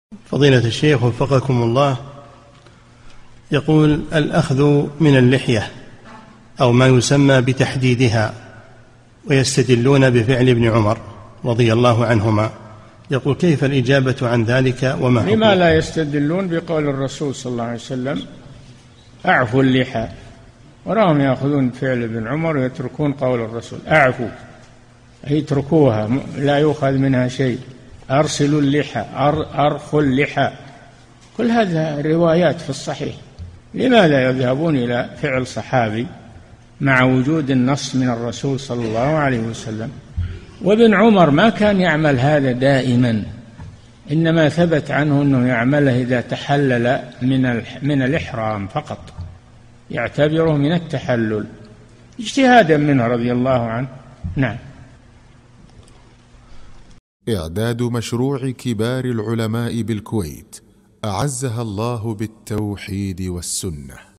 [2940 -3022] ما حكم من يحدد لحيته ويستدل بفعل ابن عمر رضي الله عنهما؟ - الشيخ صالح الفوزان
-"[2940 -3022] السؤال: الأخذ من اللحية أو ما يسمى بتحديدها ويستدلون بفعل ابن عمر رضي الله عنهما يقول كيف الإجابة عن ذلك وما حكمه؟
الجَوَابُ: لما لا يستدلون بقول الرسول ﷺ: (أعفوا اللحى) أراهم يأخذون بفعل ابن عمر ويتركون قول الرسول أعفوا، أي اتركوها لا يؤخذ منها شيء، (أرسلوا اللحى)، (أرخوا اللحى)، كل هذا روايات في الصحيح، لماذا يذهبون إلى فعل صحابي؟ مع وجود النص من الرسول ﷺ، وابن عمر ما كان يعمل هذا دائماً، إنما ثبت عنه أنه يعمله إذا تحلل من الإحرام فقط، يعتبره من التحلل، اجتهاداً منه رضي الله عنه.
للاشتراك في قناة الشيخ صالح الفوزان

للاستفسار والإقتراحات والدعم
رئيس الفريق
لمشروع كبار العلماء